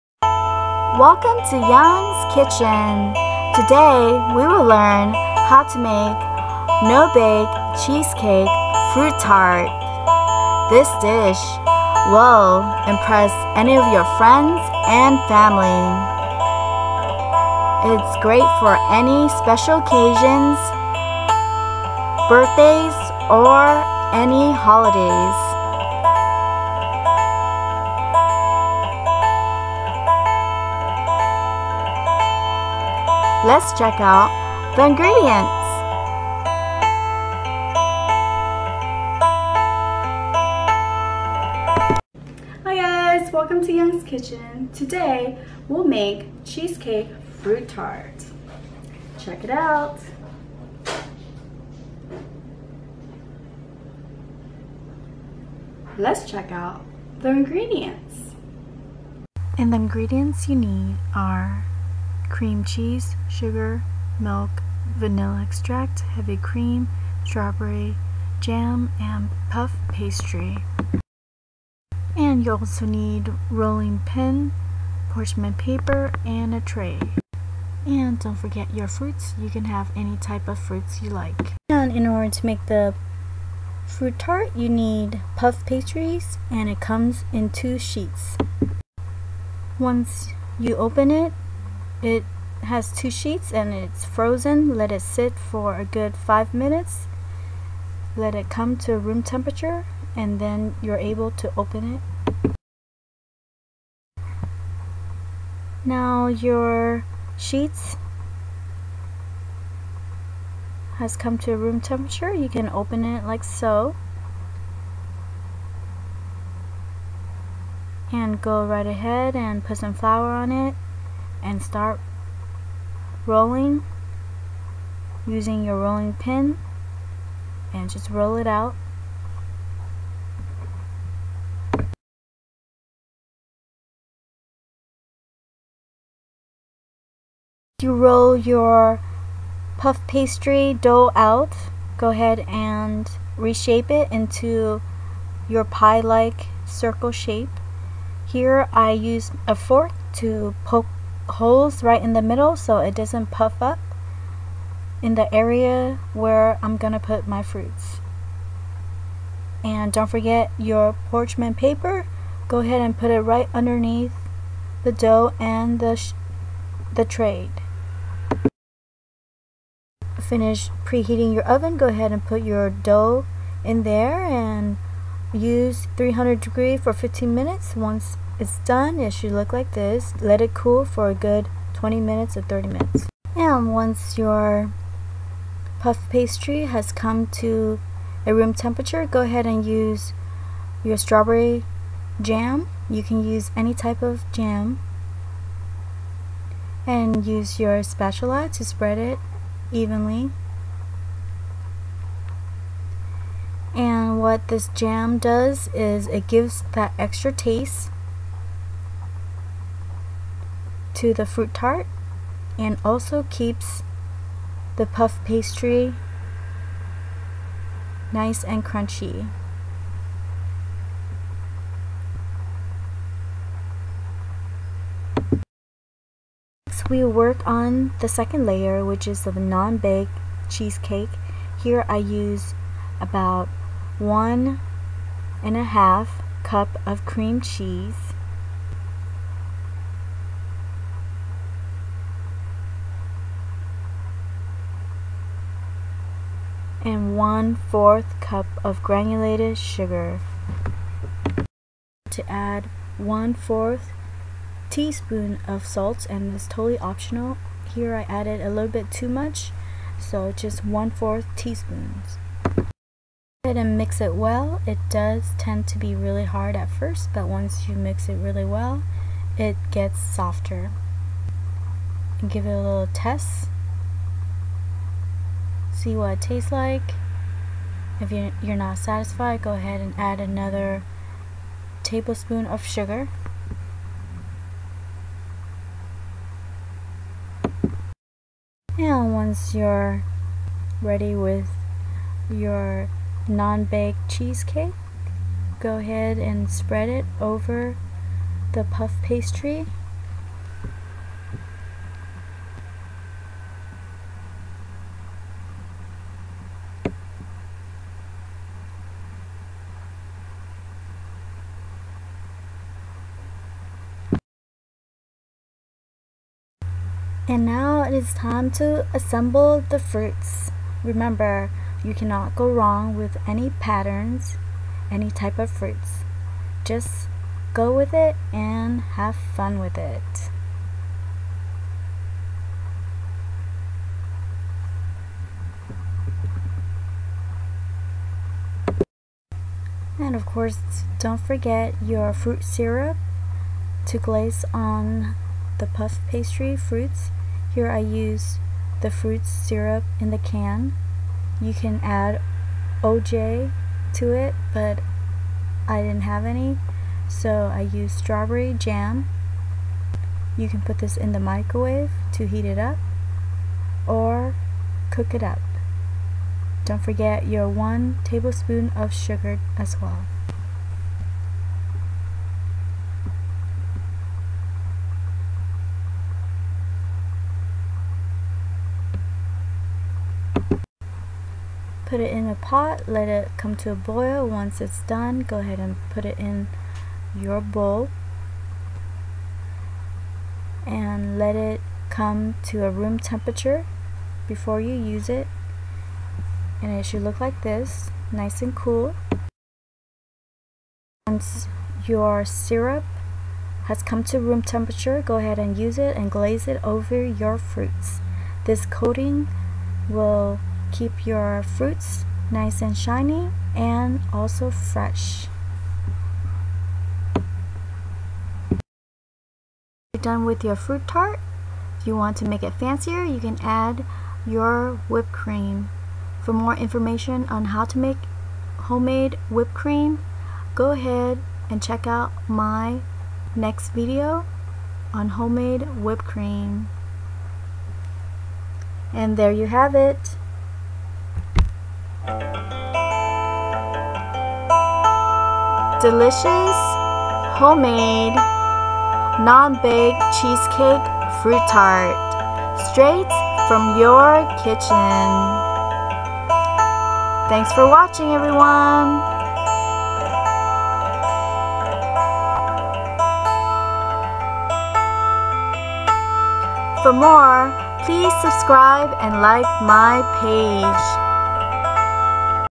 No-bake cheesecake fruit tart
Learn how to make no-bake cheesecake fruit tart in few simple steps